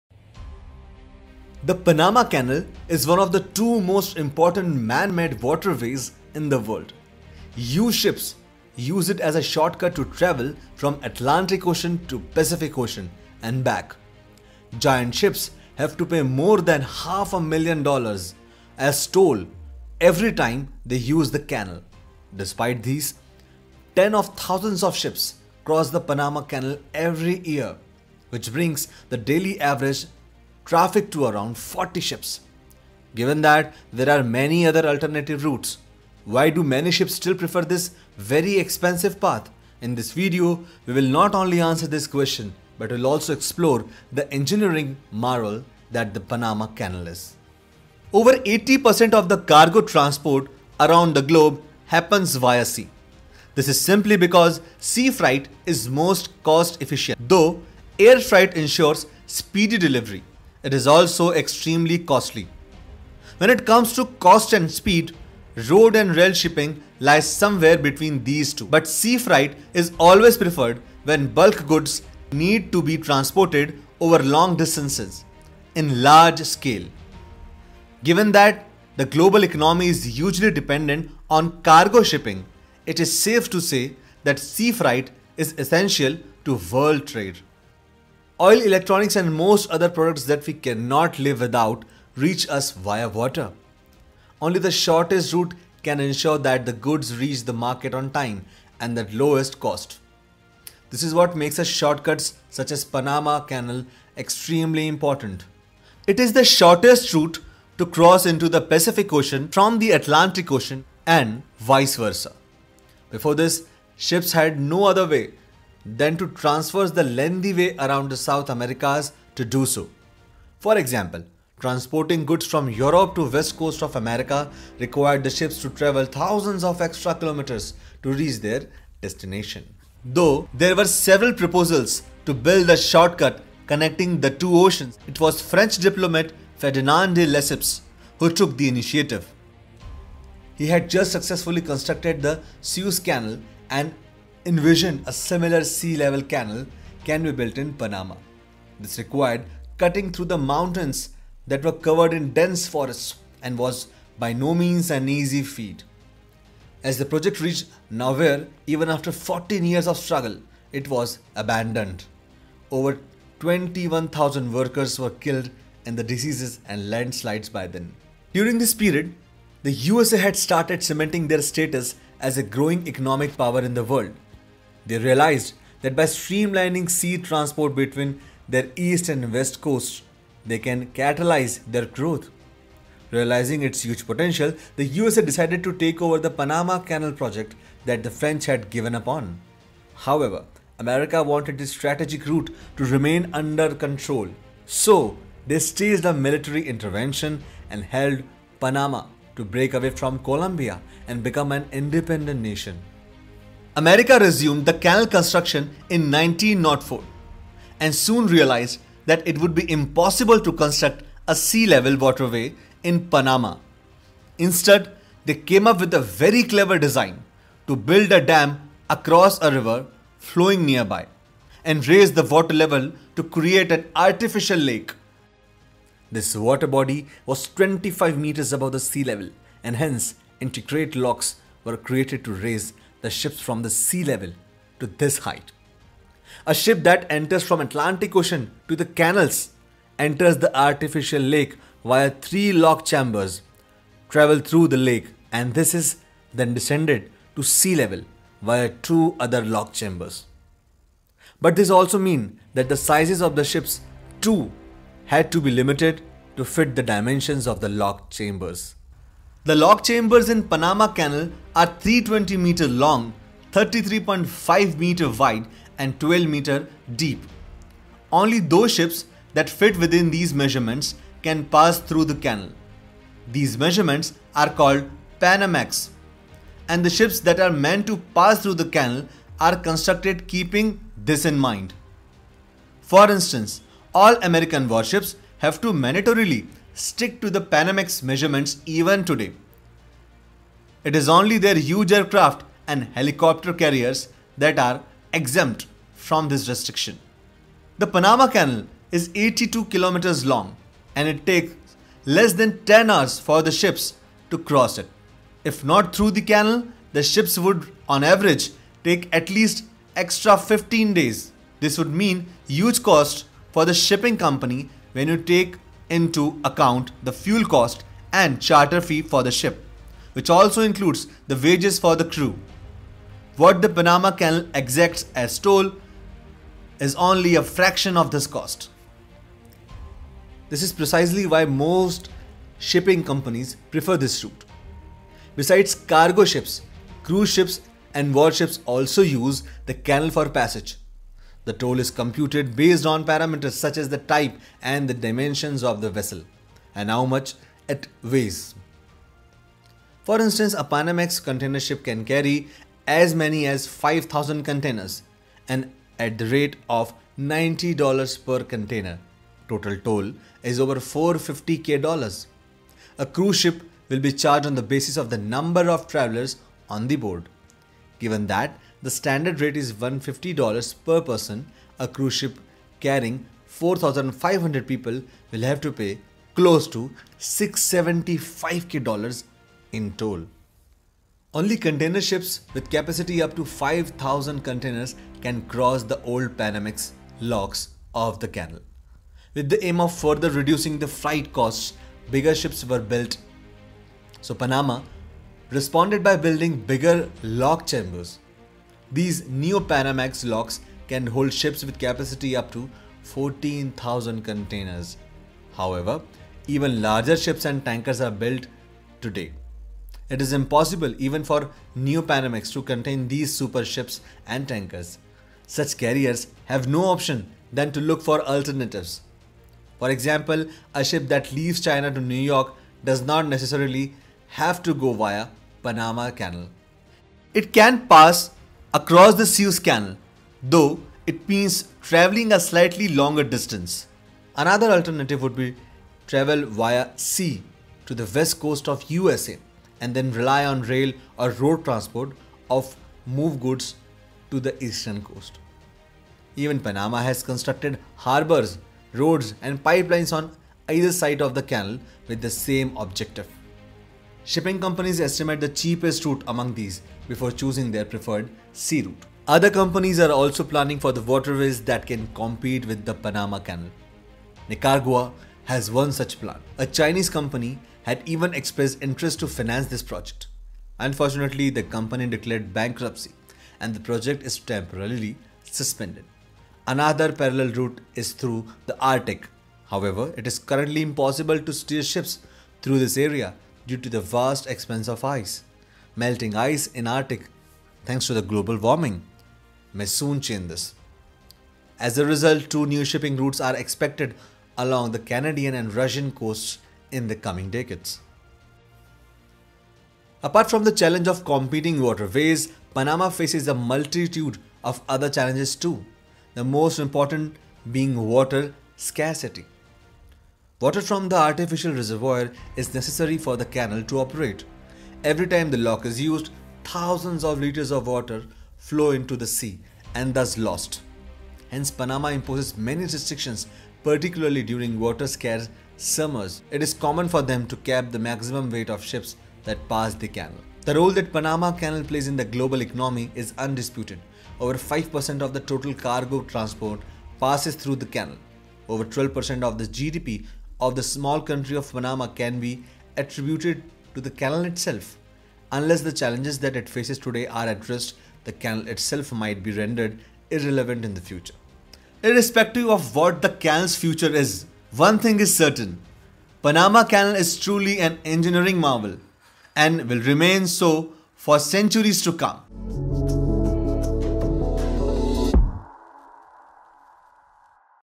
Why does it cost so much to cross Panama Canal?
Panama Canal is an engineering marvel that has helped save ships save thousands of kilometers and days in journey time. But, it costs more than a million dollars (crores of rupees) to cross the canal. Why?

Video Courtesy:
Uwebart (Published under GNU License)